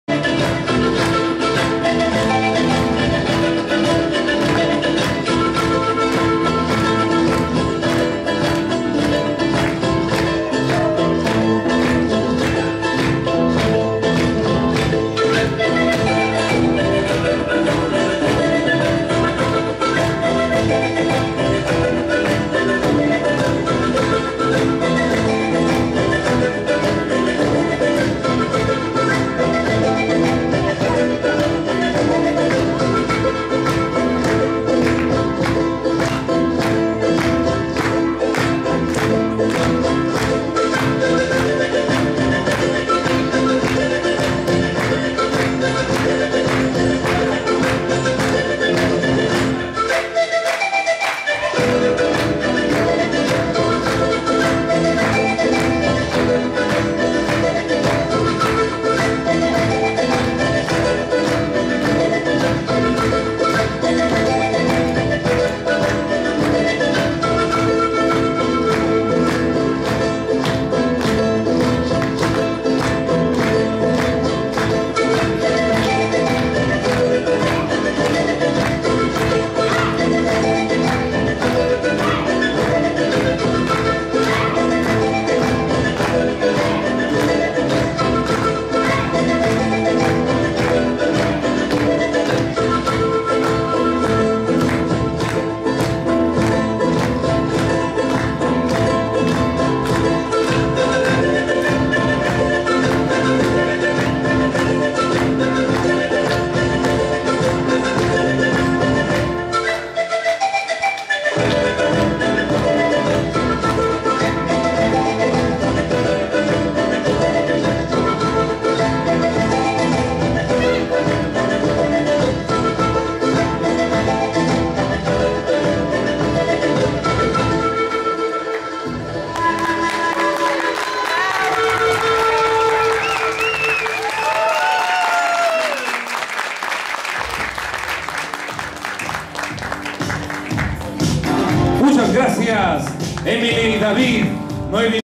Sanjuanito Expresion Inka Uk Concurso de talentos Londres 2013 Emely y David Chico
Grandes nuevos talentos que empiezan a florecer su arte!! Ganadores del Quinto Lugar!! :-)